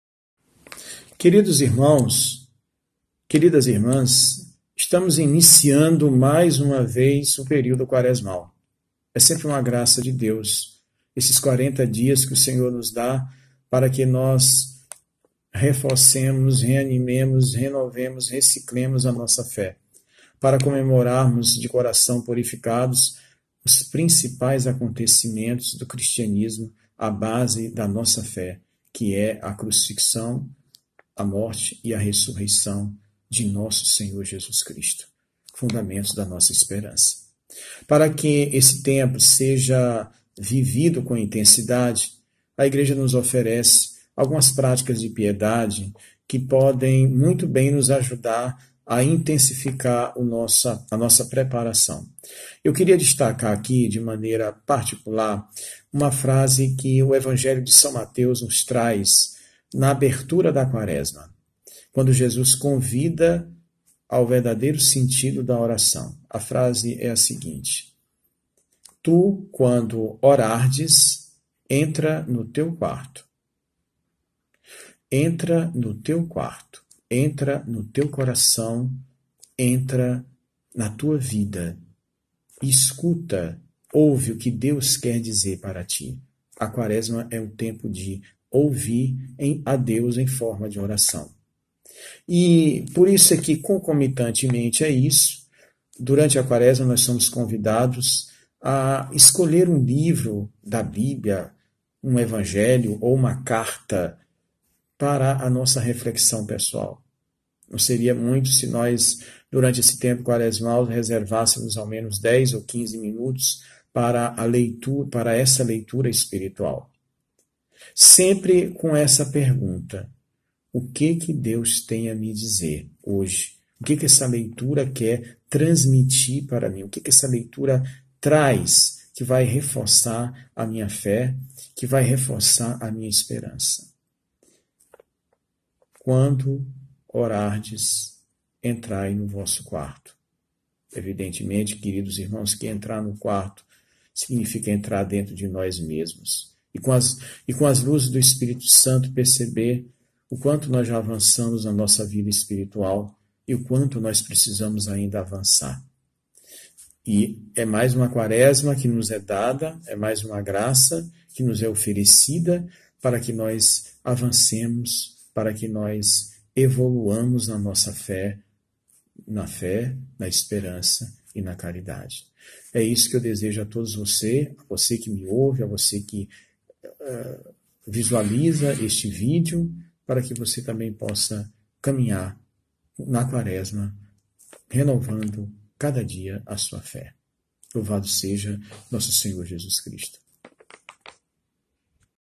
Quaresma com Pe. Paulo - Oração
Como parte da preparação para a Quaresma, Pe. Paulo nos trás a cada semana uma reflexão sobre os temas mais importantes na Quaresma.

Toda 6ª feira, ao meio-dia, um vídeo novo.